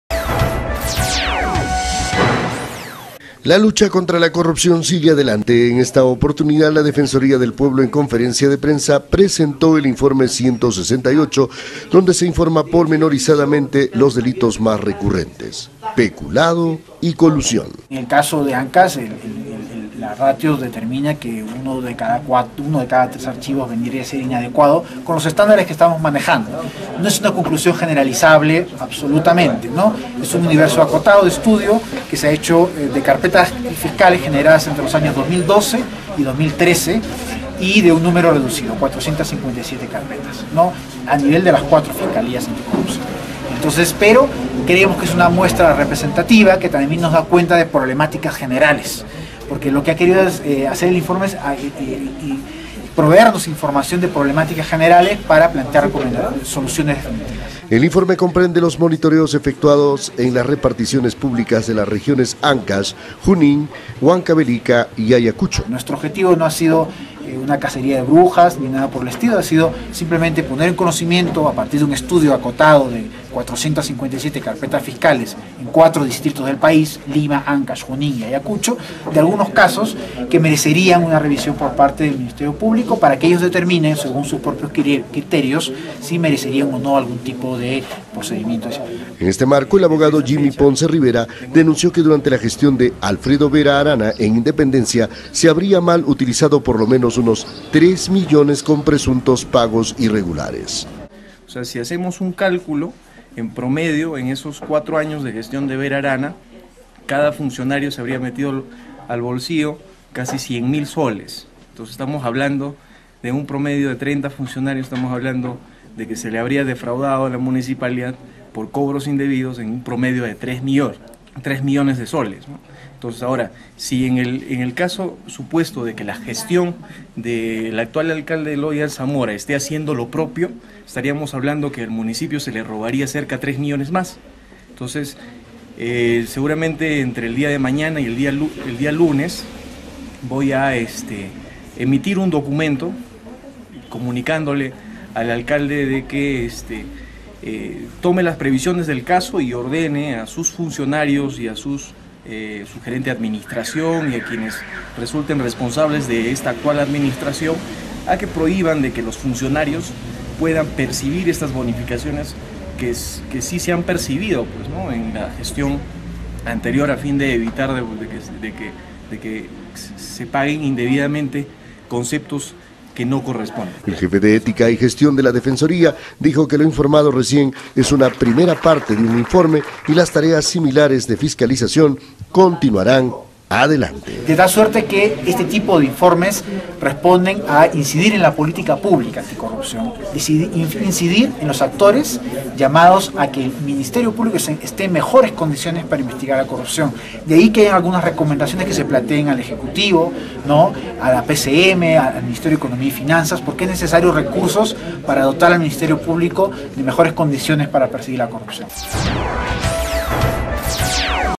corrupción en ancash prensahuaraz
Peculado y malversación de fondos en contra del Estado, son los delitos más recurrentes que se registran en Ancash, según el informe brindado por Defensoria del Pueblo. Huaraz 31 de marzo del 2015.